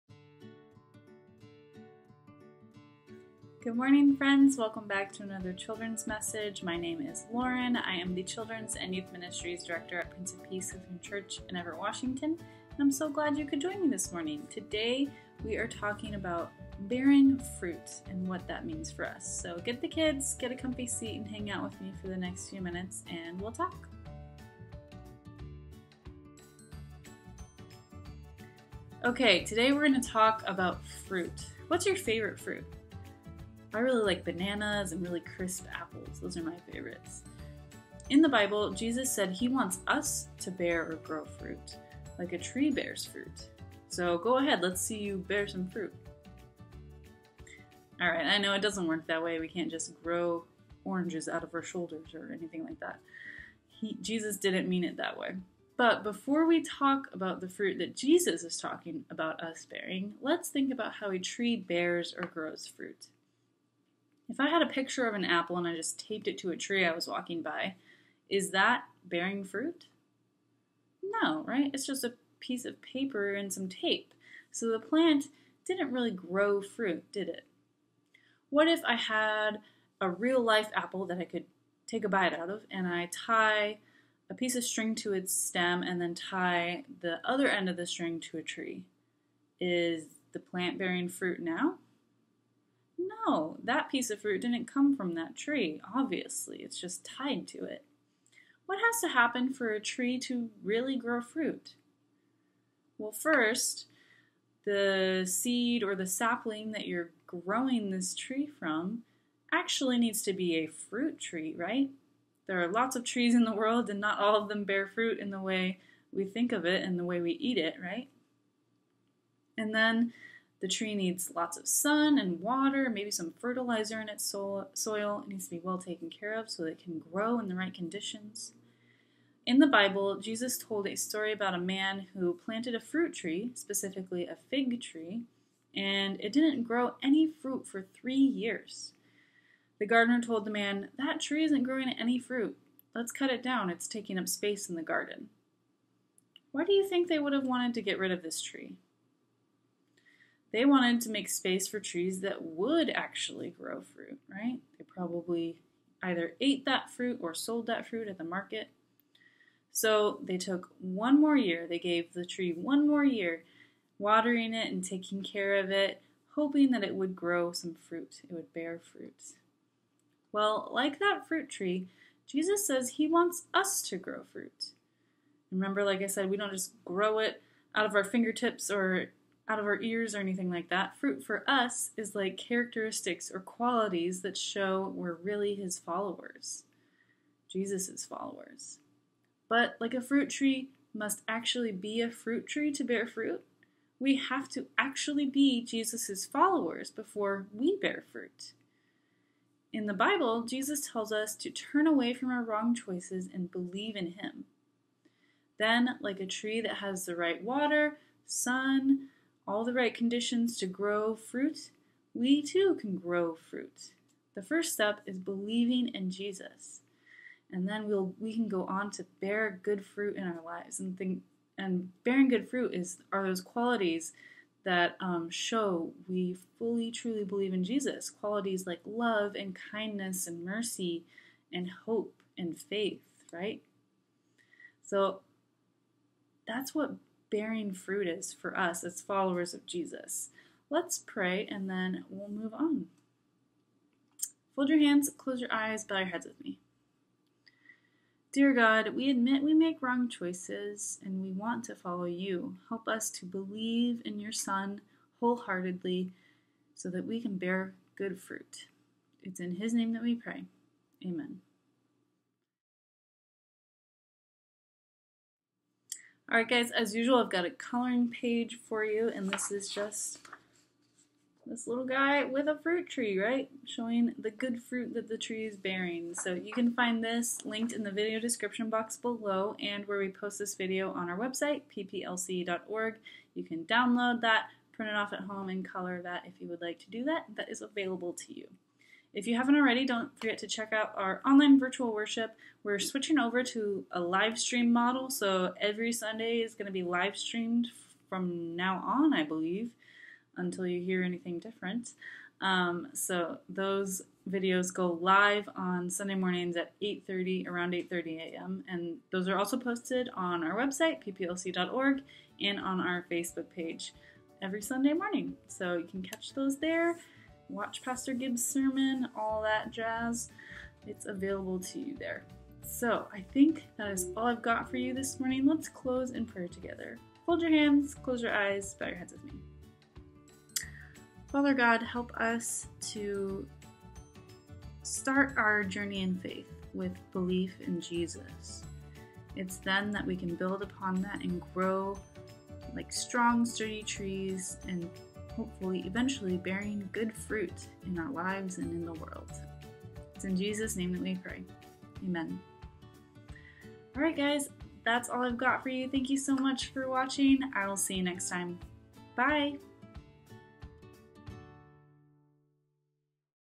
Bear Good Fruit (Children's Message for 3.20.2022)
Fruit trees are expected to grow fruit - and so are we! We should be bearing "fruit" or characteristics and qualities of true followers of Jesus! The first step is faith in him!